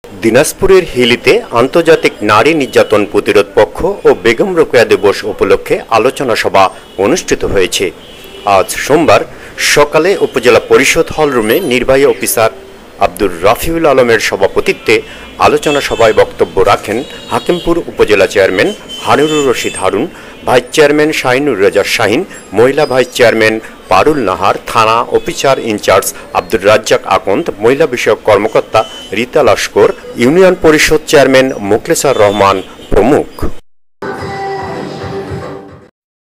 হিলিতে বেগম রোকেয়া দিবস পালিত
দিনাজপুরের হিলিতে আন্তর্জাতিক নারী নির্যাতন প্রতিরোধ পক্ষ ও বেগম রোকেয়া দিবস উপলক্ষে আলোচনা সভা অনুষ্ঠিত হয়েছে।আজ সোমবার বেলা ১১ টায় উপজেলা পরিষদ হলরুমে নির্বাহী অফিসার আব্দুর রাফিউল আলমের সভাপেিতত্বে আলোচনা সভায় বক্তব্য রাখেন, হাকিমপুর উপজেলা চেয়ারম্যান হারুন-উর-রশিদ হারুন, ভাইস চেয়ারম্যান শাহিনুর রেজা শাহীন, মহিলা ভাইস চেয়ারম্যান পারুল নাহার, থানার অফিসার ইনচার্জ আব্দুর রাজ্জাক আকন্দ, মহিলা বিষয়ক কর্মকর্তা রিতা লস্কর, খট্রামাধবপাড়া ইউনিয়ন পরিষদ চেয়ারম্যান মোকলেছার রহমান প্রমুখ।